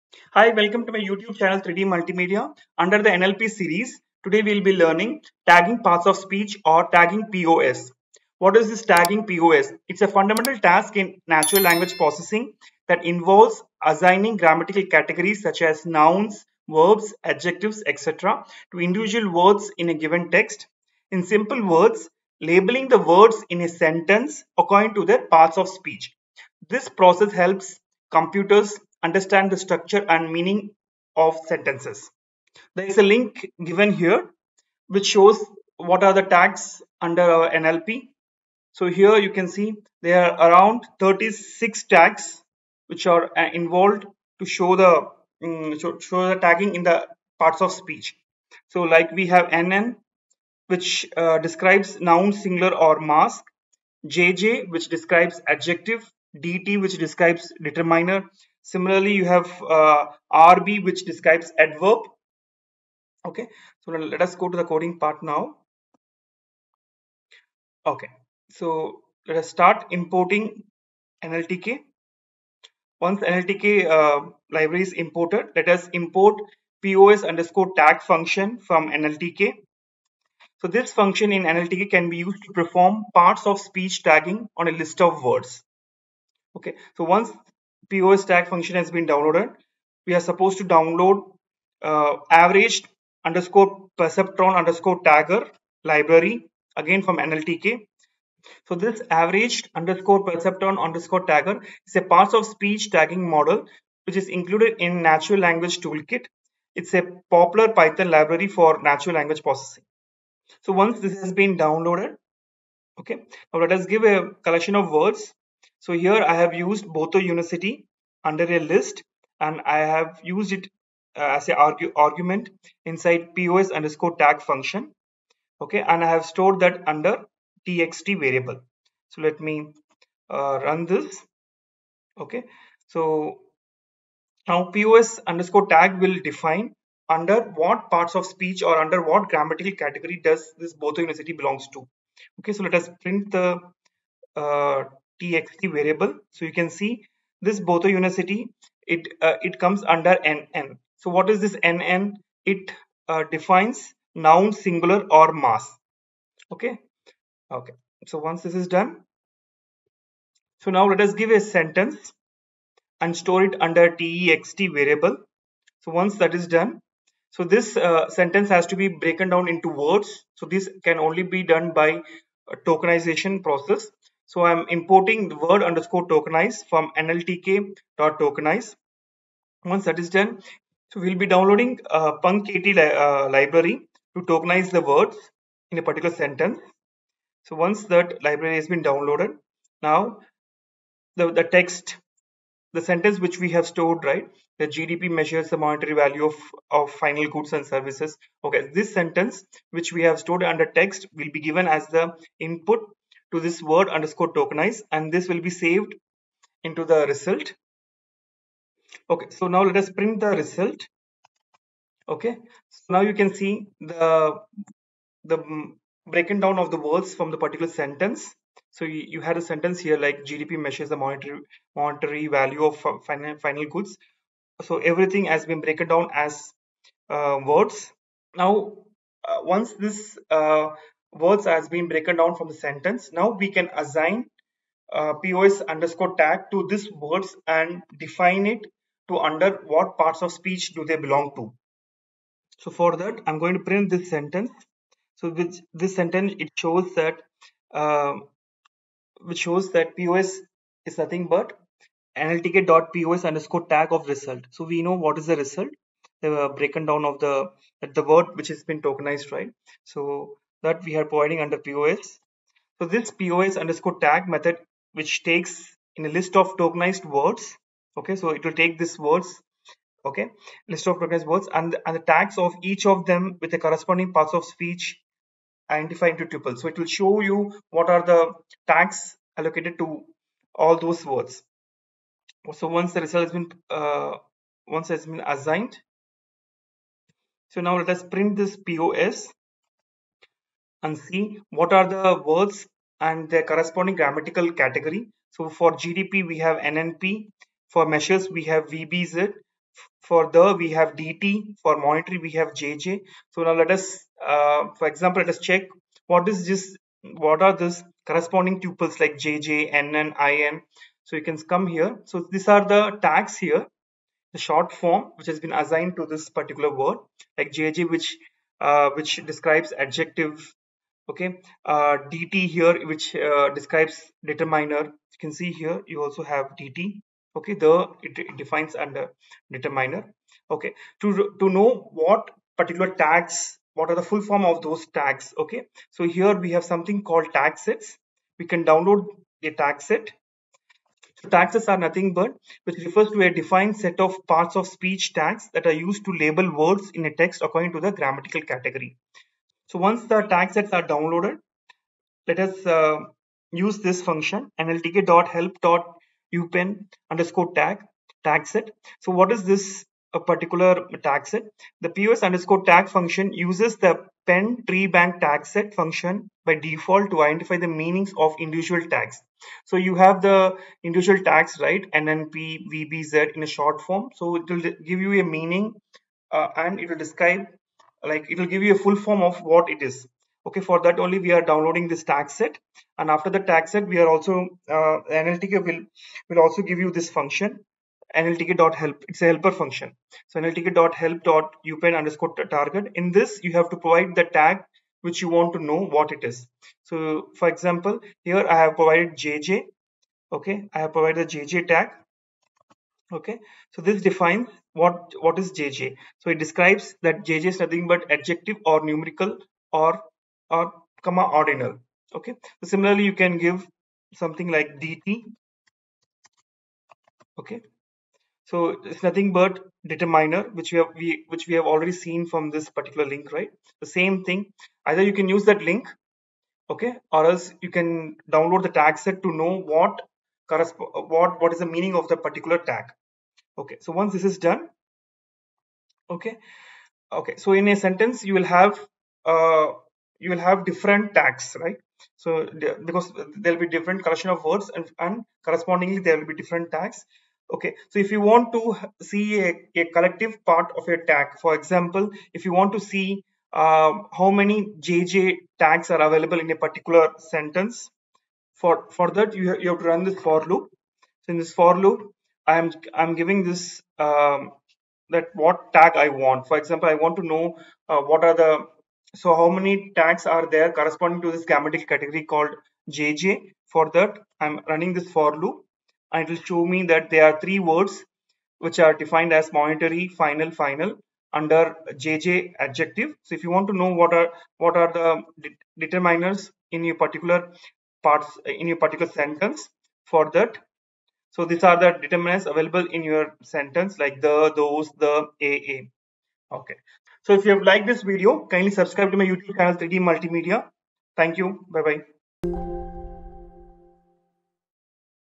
Part of Speech Tagging in NLP | POS Tagging | Natural Language Processing | Python |Tutorial 05
In this video, we will learn how to do Part of Speech (POS) Tagging  in Natural Language Processing (NLP) using Python. Tagging POS means labelling each word in a sentence corresponding to their grammatical categories.